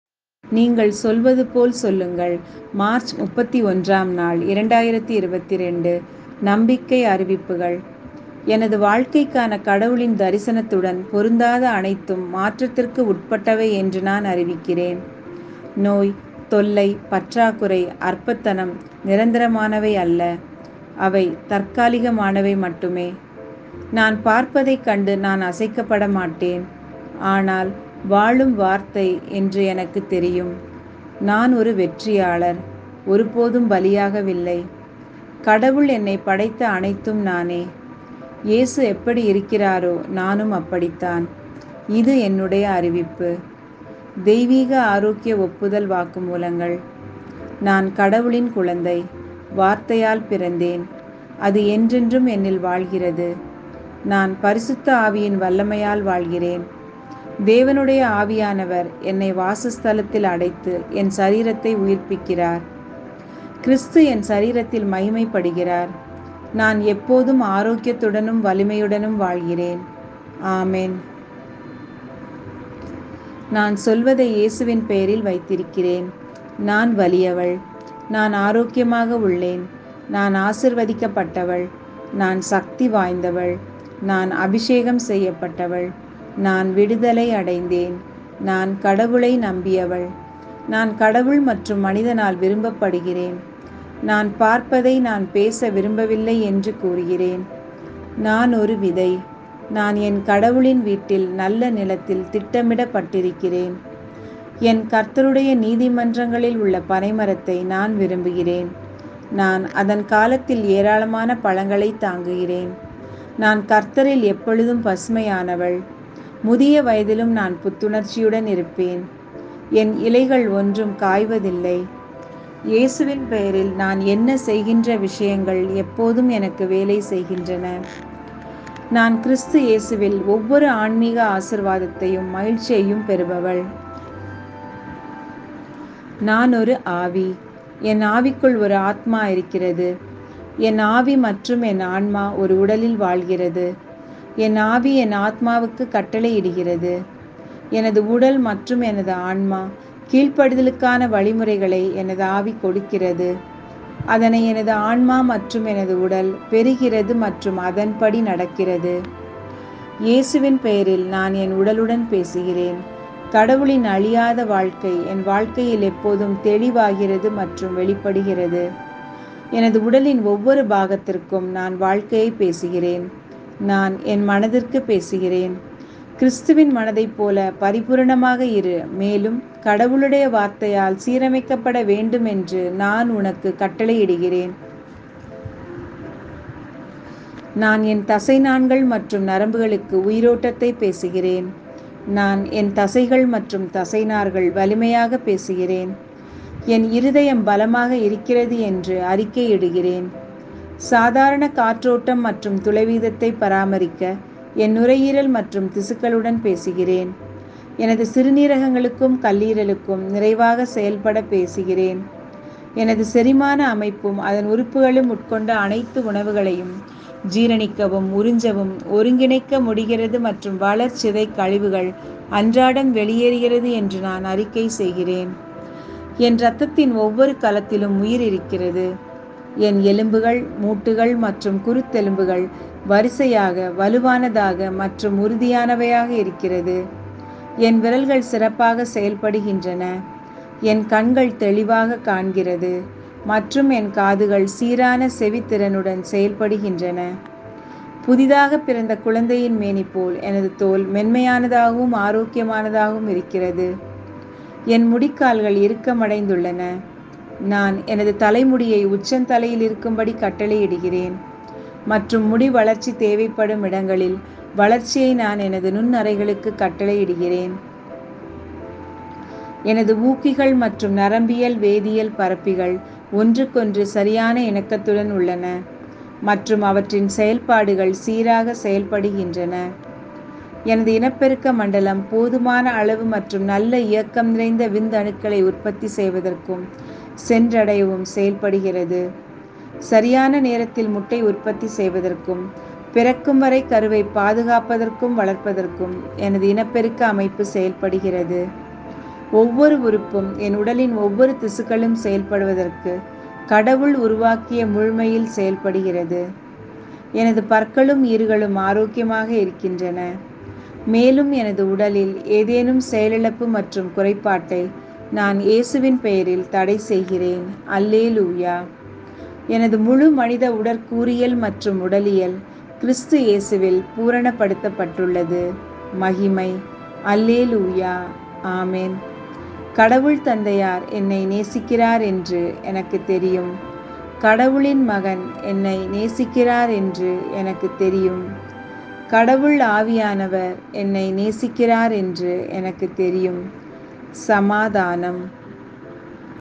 31st March 2022 Say It like You Mean It (Audio in Tamil)
நீங்கள் சொல்வது போல் சொல்லுங்கள்
31 மார்ச் 2022

 நம்பிக்கை அறிவிப்புகள்

எனது வாழ்க்கைக்கான கடவுளின் தரிசனத்துடன் பொருந்தாத அனைத்தும் மாற்றத்திற்கு உட்பட்டவை என்று நான் அறிவிக்கிறேன். நோய், தொல்லை, பற்றாக்குறை, அற்பத்தனம், நிரந்தரமானவை அல்ல. அவை தற்காலிகமானவை மட்டுமே. நான் பார்ப்பதைக் கண்டு நான் அசைக்கப்படமாட்டேன், ஆனால் "வாழும் வார்த்தை" என்று எனக்குத் தெரியும். நான் ஒரு வெற்றியாளர், ஒருபோதும் பலியாகவில்லை. கடவுள் என்னைப் படைத்த அனைத்தும் நானே. இயேசு எப்படி இருக்கிறாரோ, நானும் அப்படித்தான். இது என்னுடைய அறிவிப்பு.

 தெய்வீக ஆரோக்கிய ஒப்புதல் வாக்குமூலங்கள்

நான் கடவுளின் குழந்தை, வார்த்தையால் பிறந்தேன், அது என்றென்றும் என்னில் வாழ்கிறது. நான் பரிசுத்த ஆவியின் வல்லமையால் வாழ்கிறேன். தேவனுடைய ஆவியானவர் என்னை வாசஸ்தலத்தில் (அடைத்து) என் சரீரத்தை உயிர்ப்பிக்கிறார். கிறிஸ்து என் சரீரத்தில் மகிமைப்படுகிறார். நான் எப்போதும் ஆரோக்கியத்துடனும் வலிமையுடனும் வாழ்கிறேன் ஆமென்

  நான் சொல்வதை இயேசுவின் பெயரில் வைத்திருக்கிறேன்
 நான் வலியவன்
 நான் ஆரோக்கியமாக உள்ளேன்
 நான் ஆசீர்வதிக்கப்பட்டவன்
 நான் சக்திவாய்ந்தவன்
 நான் அபிஷேகம் செய்யப்பட்டவன்
 நான் விடுதலை அடைந்தேன்
 நான் கடவுளை நம்பியவன்
 நான் கடவுள் மற்றும் மனிதனால் விரும்பப்படுகிறேன்
 நான் பார்ப்பதை நான் பேச விரும்பவில்லை என்று கூறுகிறேன்
 நான் ஒரு விதை
 நான் என் கடவுளின் வீட்டில் நல்ல நிலத்தில் திட்டமிடப்பட்டிருக்கிறேன்
 என் கர்த்தருடைய நீதிமன்றங்களில் உள்ள பனை மரத்தை நான் விரும்புகிறேன்
 நான் அதன் காலத்தில் ஏராளமான பழங்களைத் தாங்குகிறேன்
 நான் கர்த்தரில் எப்பொழுதும் பசுமையானவன்
 முதிய வயதிலும்  நான் புத்துணர்ச்சியுடன் இருப்பேன்
 என் இலைகள் ஒன்றும் காய்வதில்லை, இயேசுவின் பெயரில் நான் என்ன செய்கின்ற
 விஷயங்கள் எப்போதும்  எனக்கு வேலை செய்கின்றன
 நான் கிறிஸ்து இயேசுவில் ஒவ்வொரு ஆன்மீக ஆசீர்வாதத்தையும் மகிழ்ச்சியையும் பெறுபவன்

  நான் ஒரு ஆவி
  என் ஆவிக்குள் ஒரு ஆத்மா இருக்கிறது.
  என் ஆவி மற்றும் என் ஆன்மா ஒரு  உடலில் வாழ்கிறது .
  என் ஆவி என் ஆத்மாவுக்கு கட்டளையிடுகிறது.  எனது உடல் மற்றும் எனது ஆன்மா கீழ்ப்படிதலுக்கான வழிமுறைகளை எனது ஆவி கொடுக்கிறது. அதனை எனது ஆன்மா மற்றும் எனது உடல் பெறுகிறது மற்றும் அதன் படி நடக்கிறது

 இயேசுவின் பெயரில், நான் என் உடலுடன் பேசுகிறேன் .
 கடவுளின்  அழியாத வாழ்க்கை  என் வாழ்க்கையில் எப்போதும் தெளிவாகிறது மற்றும் வெளிப்படுகிறது.
 எனது உடலின் ஒவ்வொரு பாகத்திற்கும் நான் வாழ்க்கையை பேசுகிறேன் .
 நான் என் மனதிற்கு பேசுகிறேன்  கிறிஸ்துவின் மனதைப் போல பரிபூரணமாக இரு,மேலும் கடவுளுடைய வார்த்தையால்  சீரமைக்கப்பட வேண்டும் என்று நான் உனக்கு கட்டளையிடுகிறேன்.
 நான் என் தசைநாண்கள் மற்றும் நரம்புகளுக்கு  உயிரோடத்தை  பேசுகிறேன்.
 நான் என் தசைகள் மற்றும் தசைநார்கள்  வலிமையாக பேசுகிறேன்.
  என் இருதயம் பலமாக இருக்கிறது என்று அறிக்கையிடுகிறேன்.
 சாதாரண காற்றோட்டம் மற்றும் துளை விகிதத்தை பராமரிக்க  என் நுரையீரல் மற்றும் திசுக்களுடன் பேசுகிறேன்.
 எனது சிறுநீரகங்களுக்கும் கல்லீரலுக்கும்  நிறைவாக செயல்பட  பேசுகிறேன்.எனது செரிமான அமைப்பும் அதன் உறுப்புகளும் உட்கொண்ட அனைத்து உணவுகளையும் ஜீரணிக்கவும், உறிஞ்சவும், ஒருங்கிணைக்க முடிகிறது மற்றும் வளர்சிதை கழிவுகள் அன்றாடம் வெளியேறுகிறது என்று நான் அறிக்கை செய்கிறேன்.
 என் இரத்தத்தின் ஒவ்வொரு கலத்திலும்  உயிர் இருக்கிறது .
 என் எலும்புகள், மூட்டுகள் மற்றும் குருத்தெலும்புகள் வரிசையாக வலுவானதாக மற்றும் உறுதியானவையாக இருக்கிறது.
 என் விரல்கள்  சிறப்பாக செயல்படுகின்றன .
 என் கண்கள்  தெளிவாக  காண்கிறது,  மற்றும்
 என் காதுகள்  சீரான செவித்திறனடன் செயல்படுகின்றன.
 புதிதாகப் பிறந்த குழந்தையின் மேனி  போல் எனது தோல்  மென்மையானதாகவும் ஆரோக்கியமானதாகவும் இருக்கிறது.
 என் முடிக்கால்கள்  இறுக்கமடைந்துள்ளன.  நான் எனது தலைமுடியை உச்சந்தலையில்  இருக்கும்படி கட்டளையிடுகிறேன், மற்றும் முடி வளர்ச்சி தேவைப்படும் இடங்களில்  வளர்ச்சியை நான்  எனது நுண்ணறைகளுக்கு கட்டளையிடுகிறேன்
 எனது ஊக்கிகள், மற்றும் நரம்பியல் வேதியியல் பரப்பிகள் ஒன்றுக்கொன்று சரியான இணக்கத்துடன் உள்ளன, மற்றும்  அவற்றின் செயல்பாடுகள் சீராக செயல்படுகின்றன.🙎🏻‍♂️எனது இனப்பெருக்க மண்டலம் போதுமான அளவு மற்றும் நல்ல இயக்கம் நிறைந்தவிந்தணுக்களை உற்பத்தி செய்வதற்கும், சென்றடையும் செயல்படுகிறது /🙍🏻‍♀️ சரியான நேரத்தில் முட்டை  உற்பத்தி செய்வதற்கும், பிறக்கும் வரை கருவைப் பாதுகாப்பதற்கும் வளர்ப்பதற்கும் எனது இனப்பெருக்க அமைப்பு செயல்படுகிறது ஒவ்வொரு உறுப்பும், என் உடலின் ஒவ்வொரு திசுக்களும் செயல்படுவதற்கு கடவுள் உருவாக்கிய முழுமையில்  செயல்படுகிறது.எனது பற்களும் ஈறுகளும் ஆரோக்கியமாக இருக்கின்றன. மேலும் எனது உடலில் ஏதேனும் செயலிழப்பு மற்றும் குறைபாட்டை நான் தடைசெய்கிறேன்
 இயேசுவின் பெயரில்.  ஹல்லெலூஜா!  எனது முழு மனித உடற்கூறியல் மற்றும் உடலியல் கிறிஸ்து இயேசுவில் பூரணப்படுத்தப்பட்டுள்ளது மகிமை அல்லேலூஜா ஆமென்.

 * கடவுள் தந்தையார், என்னை நேசிக்கிறார் என்று எனக்குத் தெரியும் .
 * கடவுளின் மகன், என்னை நேசிக்கிறார், என்று எனக்குத் தெரியும் *
  கடவுள் ஆவியானவர் என்னை நேசிக்கிறார், என்று எனக்குத் தெரியும் .

  சமாதானம்